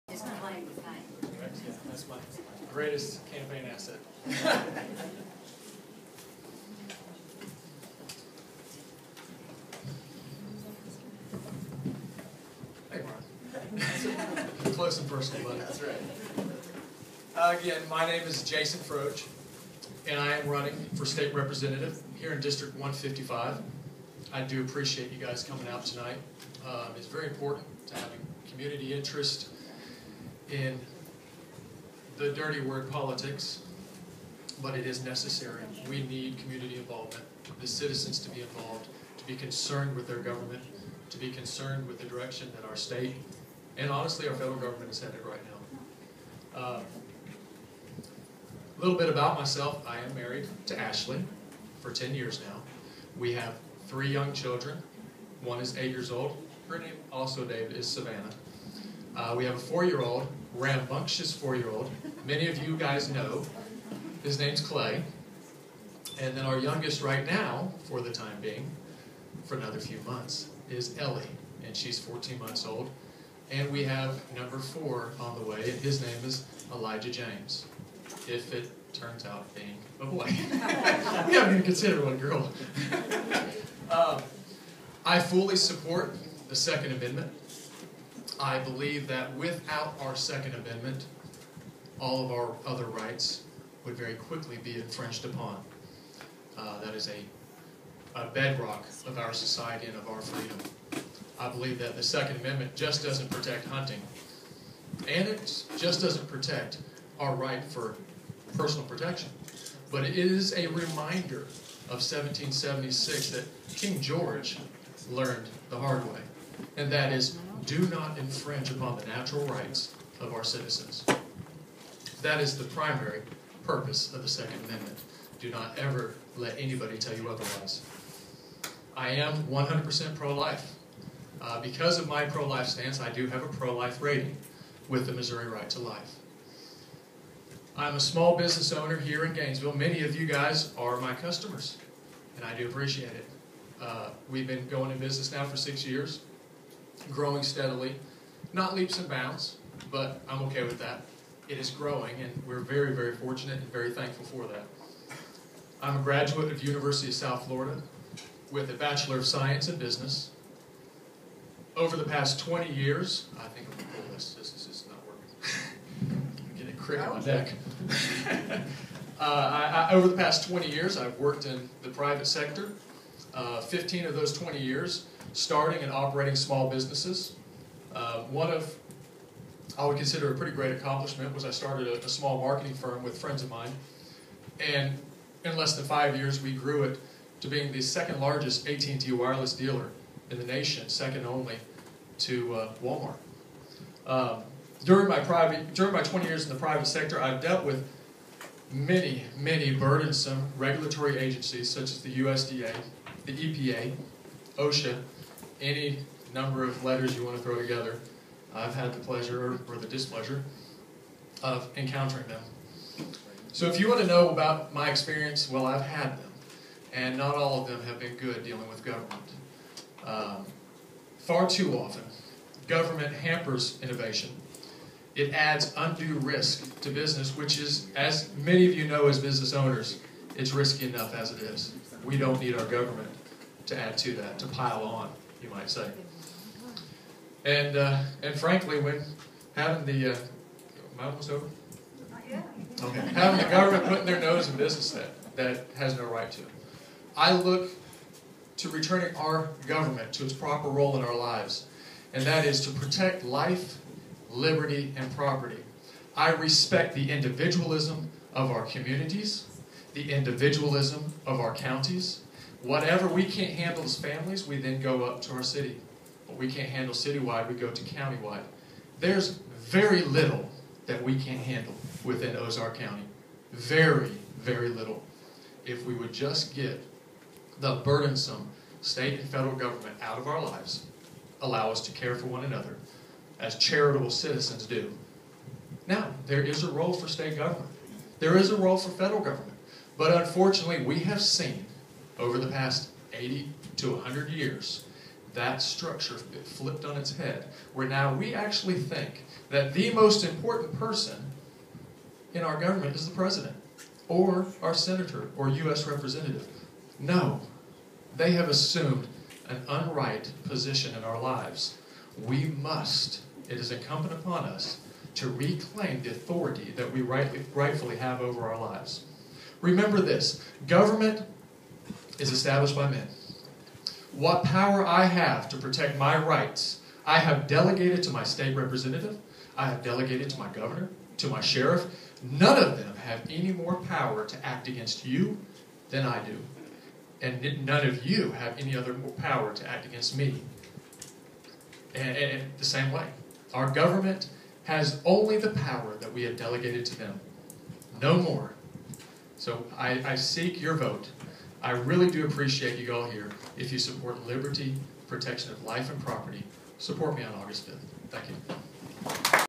Jason Frodge candidate District 155 Mo. House of Representa
Meet the candidate night Senior Center Gainesville, Mo. 7/18/14